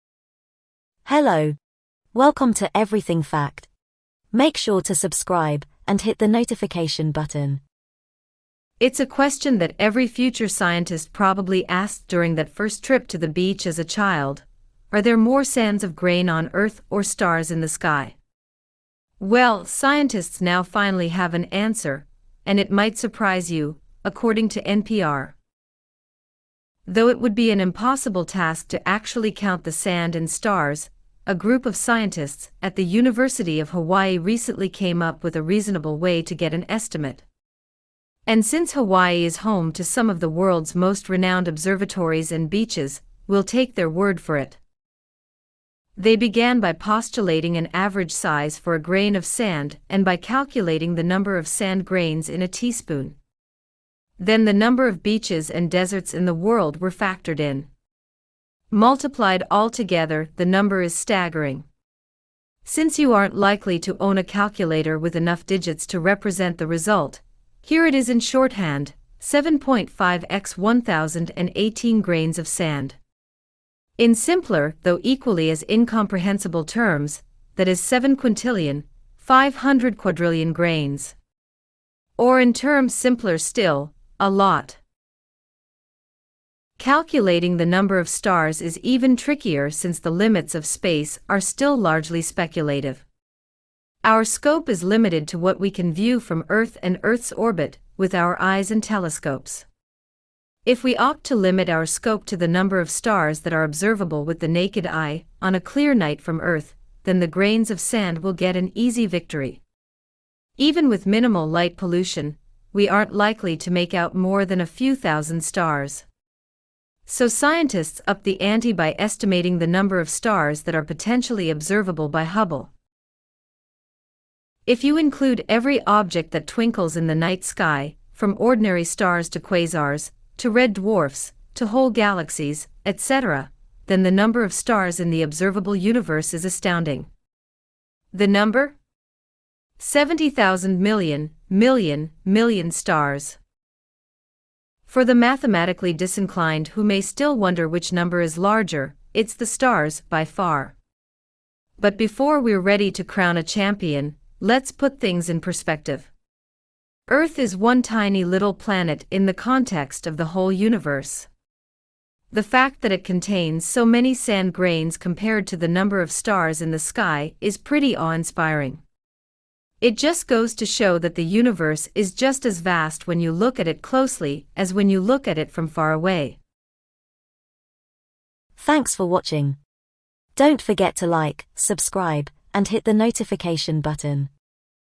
Why There are more stars in the universe than sand on the earth ?
There are more stars in the universe than grains of sand on the earth and the number of water molecules in ten drops of water is equal to all the stars in the known universe.